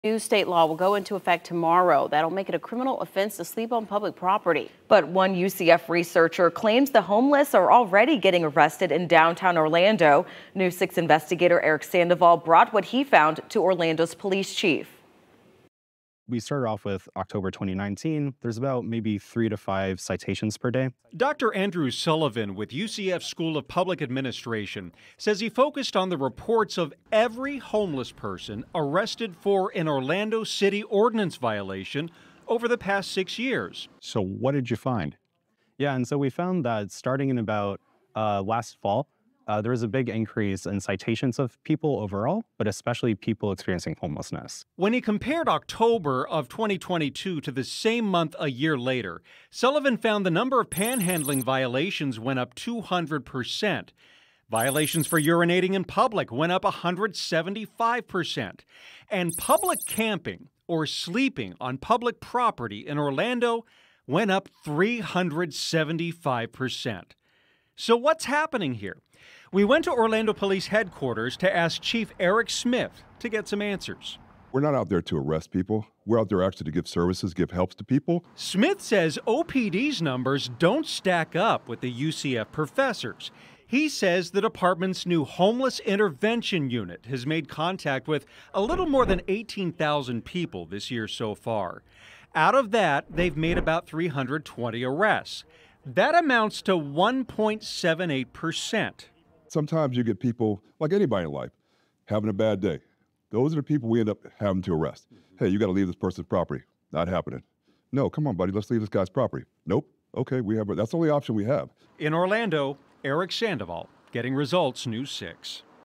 Researcher: Orlando homeless arrests up significantly ahead of new law banning sleeping in public
A University of Central Florida researcher claims the homeless population in downtown Orlando already faced a rising number of arrests ahead of a new state law that will ban sleeping on public property.